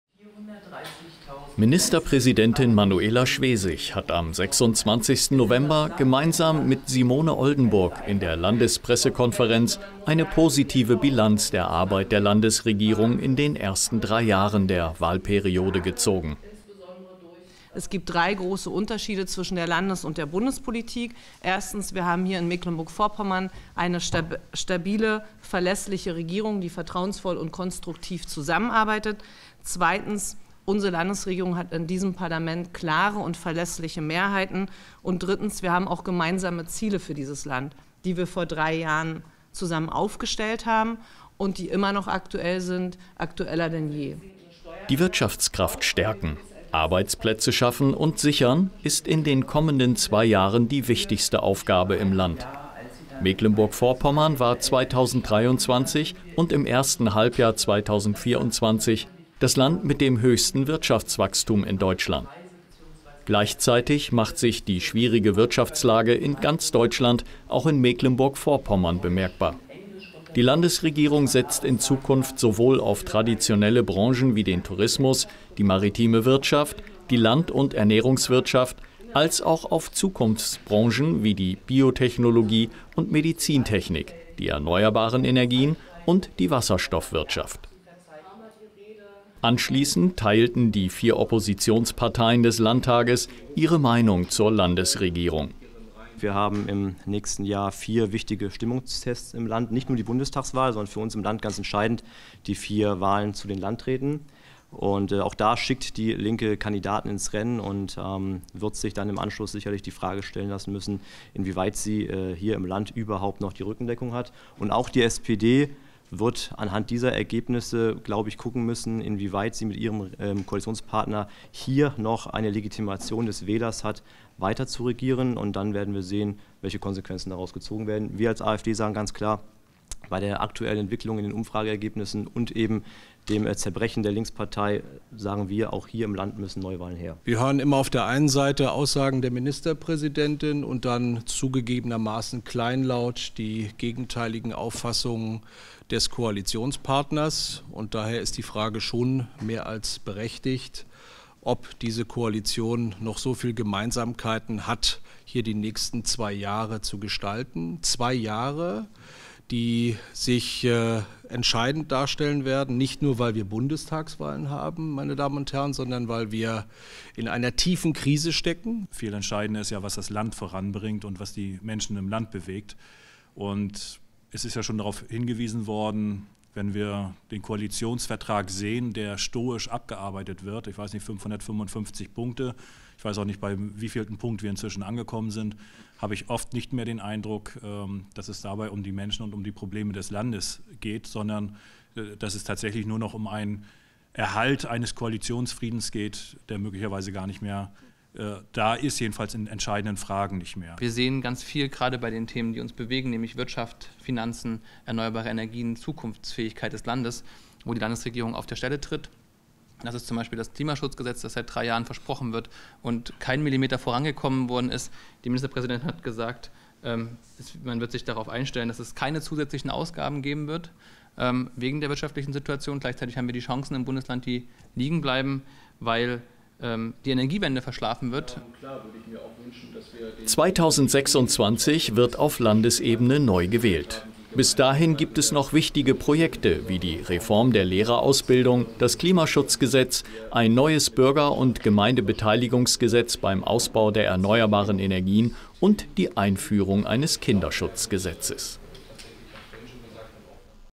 Landesregierung zieht positive Drei-Jahres-Bilanz - Opposition sieht das anders und übt Kritik
Minister­präsi­dentin Manuela Schwe­sig (SPD) und ihre Stellvertreterin Simone Oldenburg (Die Linke) haben am 26. No­vem­ber gemeinsam  in der Landes­presse­kon­ferenz eine positive Bilanz der Arbeit der Landes­regierung in den ersten drei Jahren der Wahl­periode gezogen. Anschließend kritisierten die Oppositionsparteien diese Bilanz.